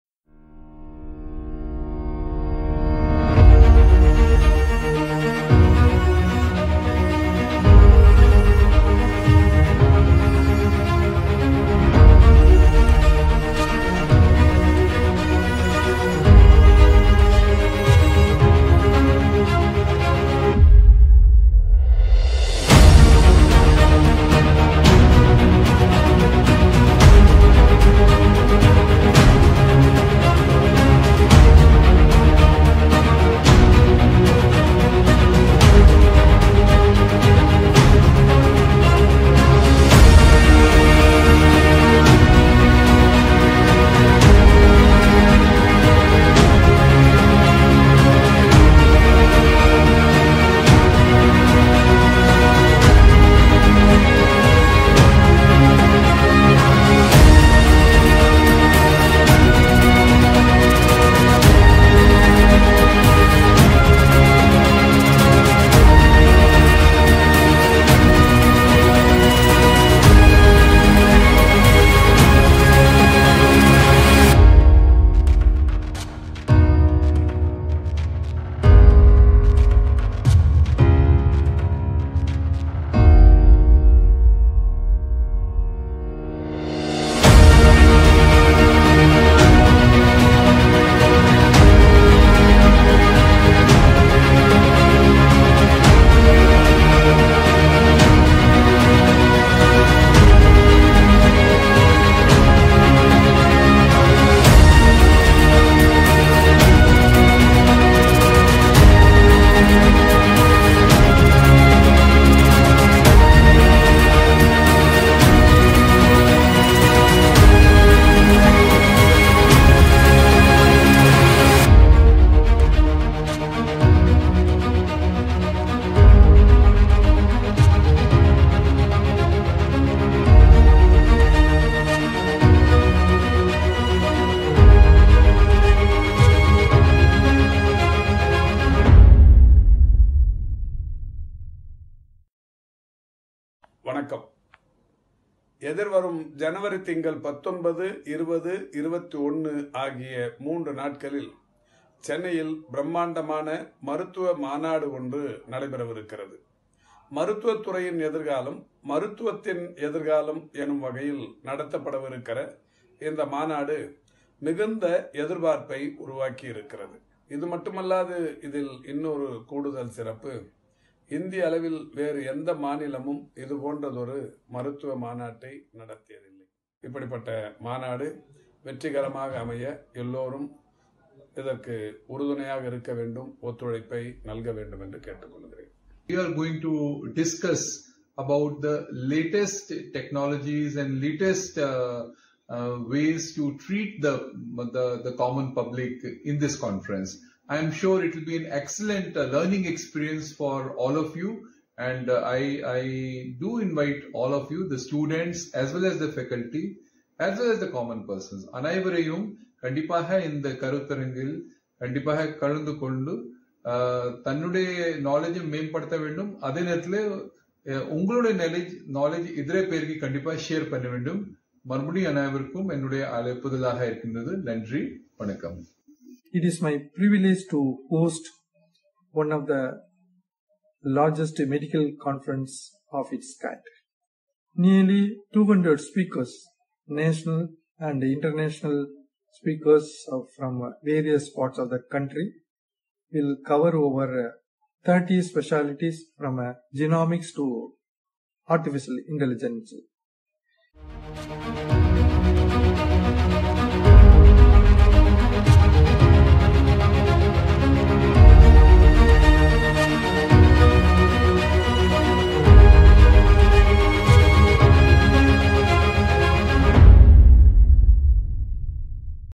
Kalaignar Centenary International Medical Conference - Future of Medicine - 19th to 21st Jan 2024
Official Video of one of the Largest International Medical Conference in India, happening in Chennai.

Organized by the Department of Health, Medical Education and Family Welfare, Government of Tamil Nadu jointly with The Tamil Nadu Dr. M.G.R. Medical University at Chennai Trade Centre, Nandambakkam.

With 200+ National Speakers and 25+ International Experts covering 30+ Specialties over a span of 3 days, BE a part of this Mega Bonanza; A once-in-a-lifetime learning opportunity.

00:02:00 - Accreditation by Tamil Nadu Medical Council, Tamil Nadu Dental Council & Tamil Nadu Nurses & Midwives Council

00:02:07 - Surgical Skill Training Programme in Endotrainer (Laparoscopic) and Basic Suturing

00:02:15 - Public Awareness Program & Mammogram Screening

00:02:20 - REGISTER HERE

00:02:23 - Address by Thiru. Ma. Subramanian, Honourable Minister of Health, Medical Education and Family Welfare, Government of Tamil Nadu

00:03:06 - Address by Thiru Gagandeep Singh Bedi, IAS, Additional Chief Secretary to Government, Department of Health, Medical Education and Family Welfare, Government of Tamil Nadu

00:03:46 - Address by Prof. Dr. K. Narayanasamy, Vice-Chancellor of The Tamil Nadu Dr. M.G.R. Medical University